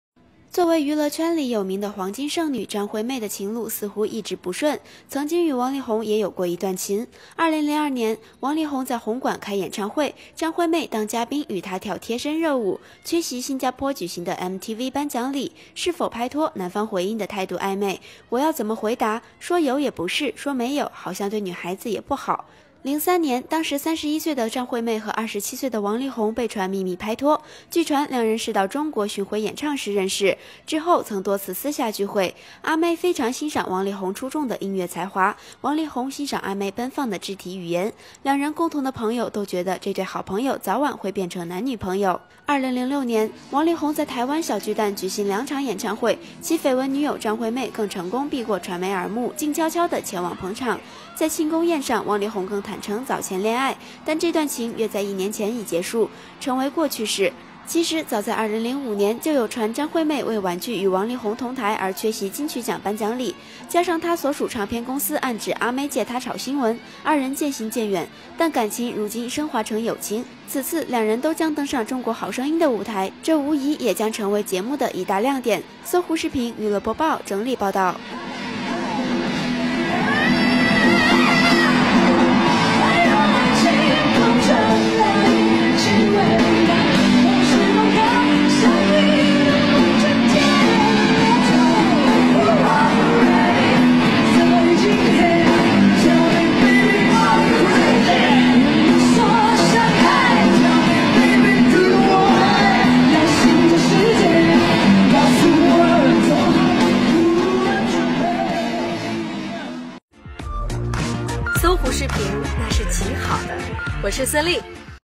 《搜狐娱乐》张惠妹情史揭秘：曾与王力宏传绯闻
【搜狐视频娱乐播报】报道 作为娱乐圈里有名的"黄金剩女"张惠妹的情路似乎一直不顺,曾经与王力宏也有过一段情。2002年，王力宏在红馆开演唱会，张惠妹当嘉宾与他跳贴身热舞，缺席新加坡举行的MTV颁奖礼。是否拍拖?男方回应的态度暧昧："我要怎么回答?说有也不是，说没有，好像对女孩子不好。"03年，当时31岁的张惠妹和27岁的王力宏被传秘密拍拖。据传两人是到中国巡回演唱时认识，之后曾多次私下聚会，阿妹非常欣赏王力宏出众的音乐才华，王力宏欣赏阿妹奔放的肢体语言。两人共同的朋友都觉得这对"好朋友"早晚会变成男女朋友。这年年底，黑人透露王力宏看完陈凯歌的电影《和你在一起》后，突然哭了，因为他对台词"我很寂寞，我只有音乐"感同身受。黑人以此为论据，得出王力宏果然只是张惠妹好朋友的结论："我本来很想问清楚，他和阿妹的关系，不过，力宏的告白很清楚了，我就没有再问。"2006年王力宏在台湾小巨蛋举行的两场演唱会，其绯闻女友张惠妹更成功避过传媒耳目，静悄悄地前往捧场。在庆功宴上，王力宏更坦承早前恋爱，但这段情约在一年前已结束，成为过去式!其实早在2005年就有传张惠妹为婉拒与王力宏同台而缺席金曲奖颁奖礼。加上他所属唱片公司暗指阿妹藉他炒新闻，2人渐行渐远，但感情如今已升华为友情，此次两人都将登上中国好声音的舞台，这无疑也会成为节目的一大亮点。